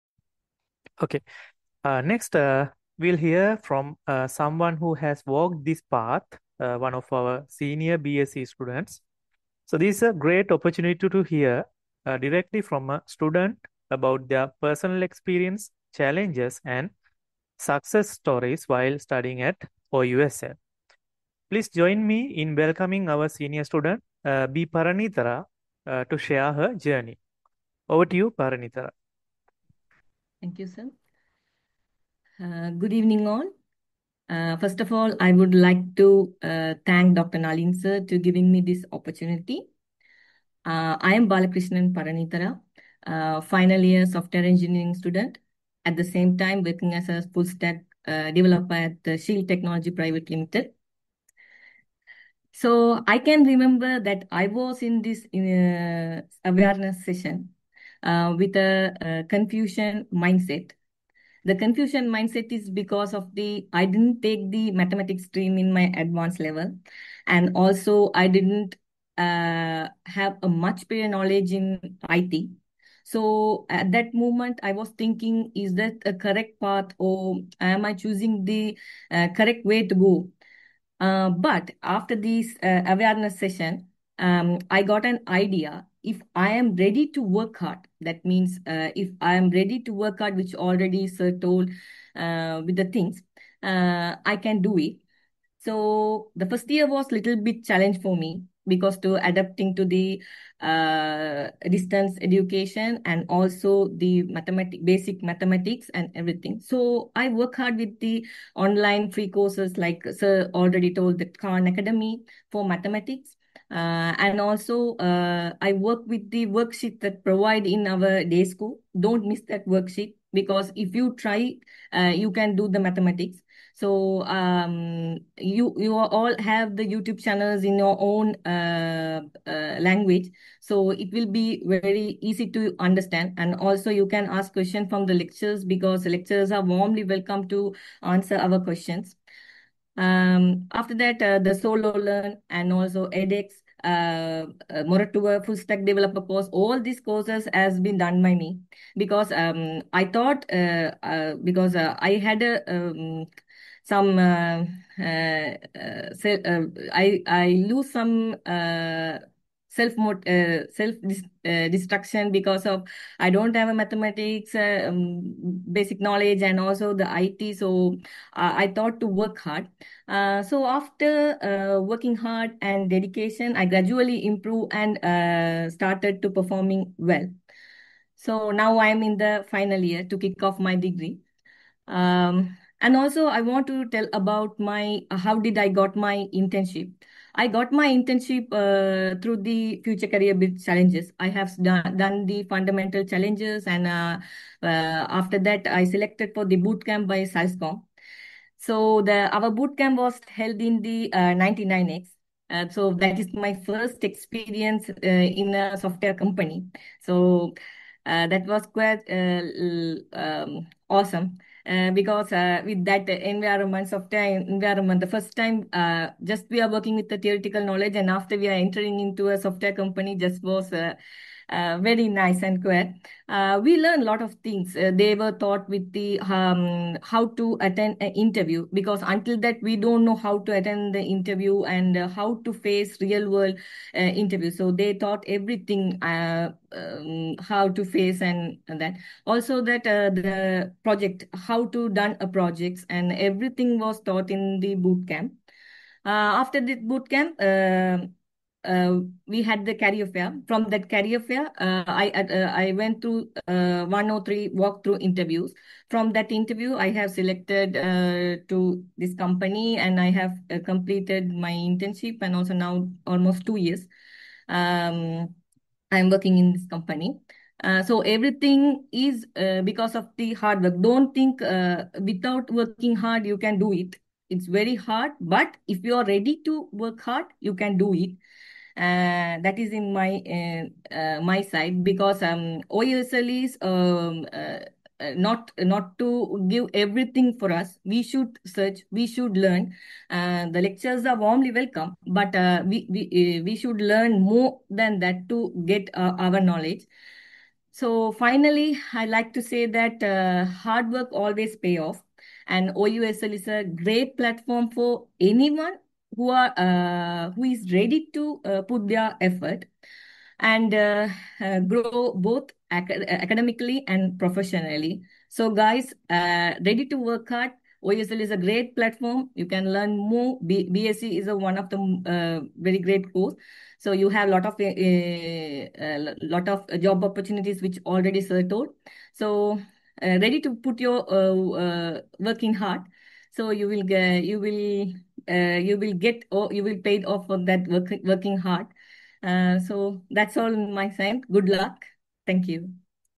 Message from a senior student in the BSE@OUSL
This message is from B. Paraneethara senior student in the Bachelor of Software Engineering (BSE) program at the Open University of Sri Lanka (OUSL) to those interested in applying to the program for the academic year 2025/26.

She joined the BSE program in 2021/22 with 3 passes in the Common Stream of the Advanced Levels (no Combined Mathematics) and no knowledge of programming.  She has succeeded in the program by working hard to make up for the weaknesses in her background by following many optional programs before and during the Induction Period - including the Future Careers Bridge.

She was invited to a Boot Camp conducted by the Sri Lanka Association of Software and Service Companies (SLASSCOM) due to her completion of the Future Careers Bridge.  She obtained an internship at Shield Technologies Pvt. Ltd. through the Career Fair organized by SLASSCOM.  She is now employed at the same company as a Backend Developer following one year of internship.

The message was recorded during the Awareness Session held on the 25th of October, 2025.

- DEADLINE EXTENDED! Applications close 19th of December, 2025